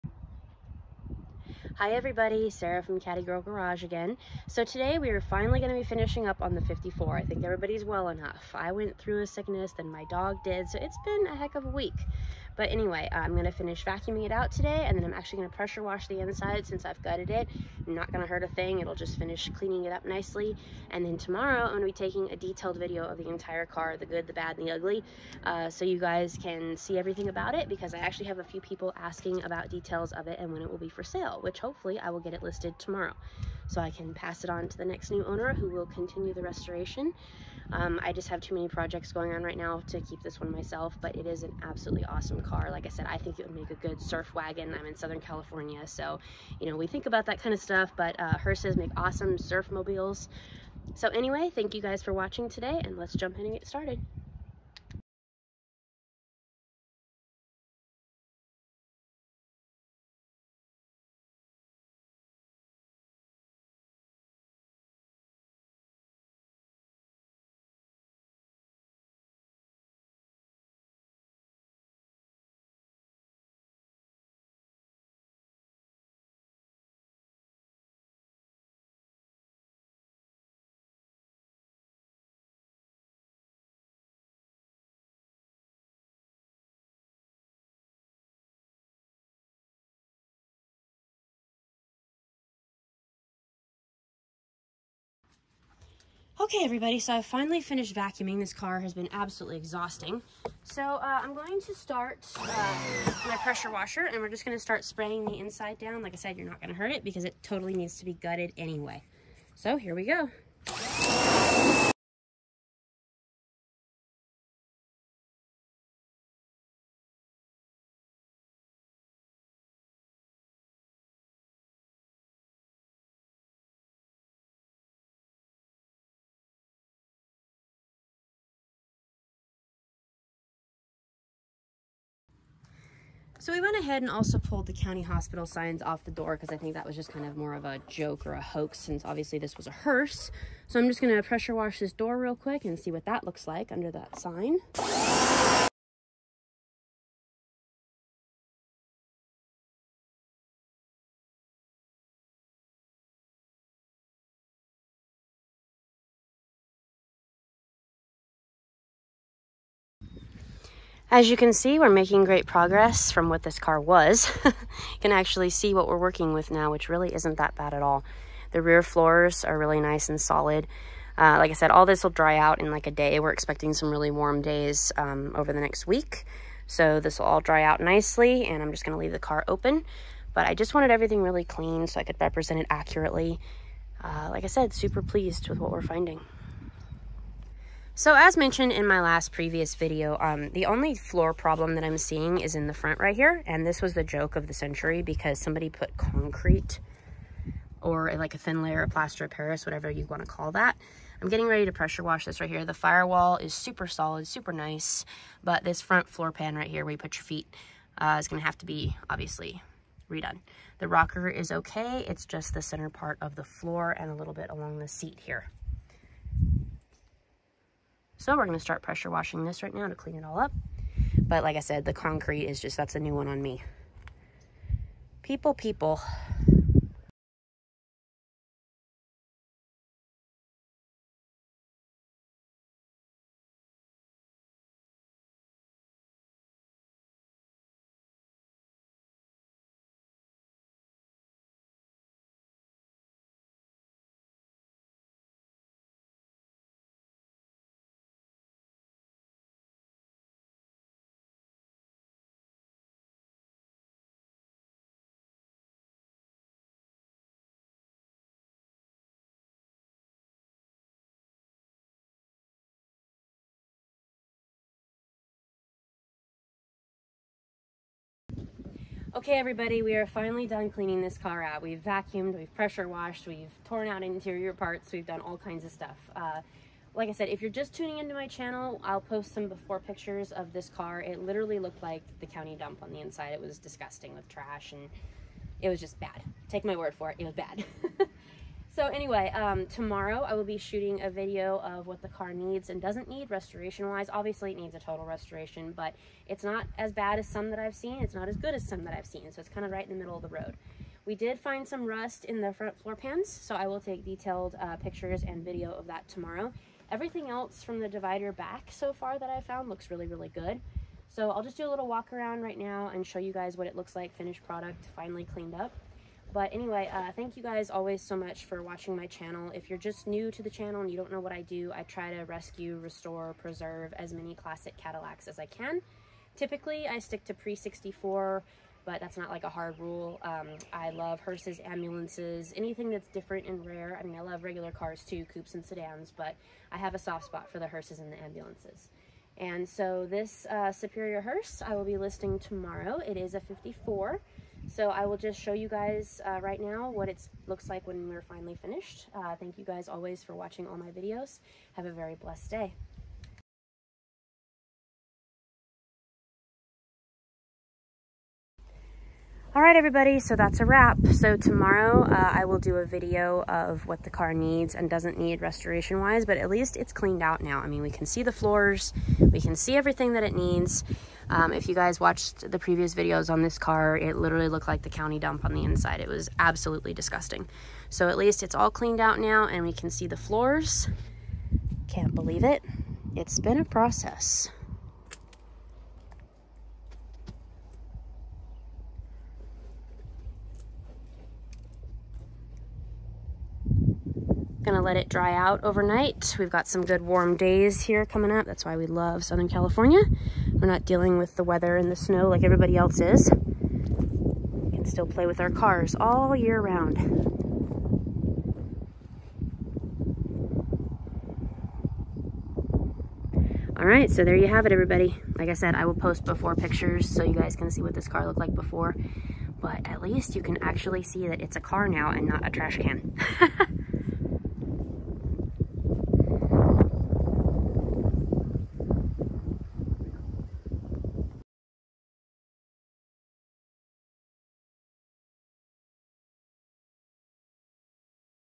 The 54 is finally done!
The 54 is finally done getting cleaned! I’ll be posting the car on eBay tomorrow for sale!